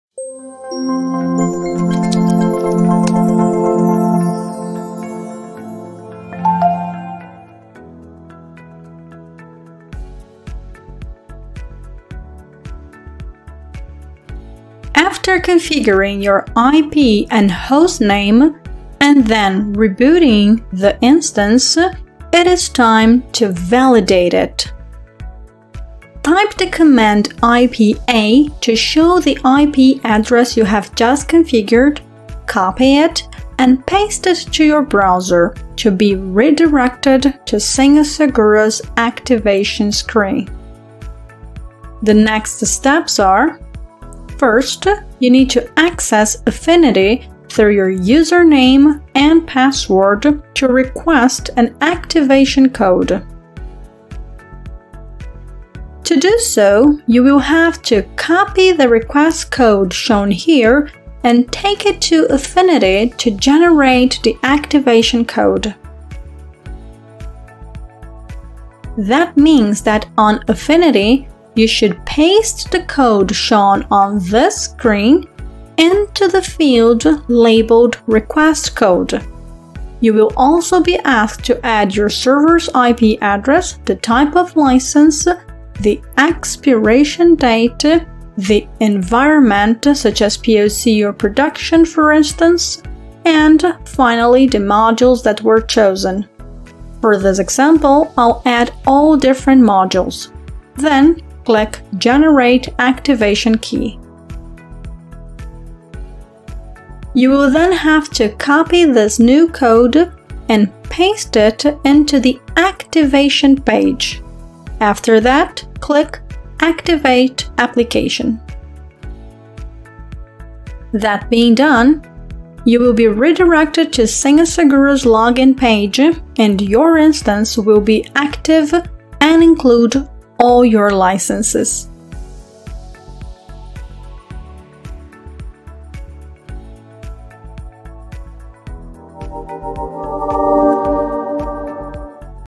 How to activate a senhasegura license
In this video, you will check how to activate a senhasegura license. This procedure is required after the deployment of senhasegura and the configuration of the  IP and Hostname:

1. In your terminal type, the command ip a, and use the address IP to access senhasegura via the web.
2. Insert the IP in the browser, you will redirect you to the senhasegura activation screen, and copy the request code.
3. Access the Affinity portal, in the tab ‘Request code’ insert the ‘Hostname or Server’s IP’, and select the type of license, expiration date, and Environment.
4. In ‘Request code (generated by Orbit)*:’ paste the request code copied.
5. Select which senhasegura modules will be activated.
6. Then click on ‘Generate activation key’ and copy the ‘Activation key’.
7. In the senhasegura activation, the screen pastes the activation key and clicks on Activate application.
8. You will be redirected to the senhasegura login page and the system will be available.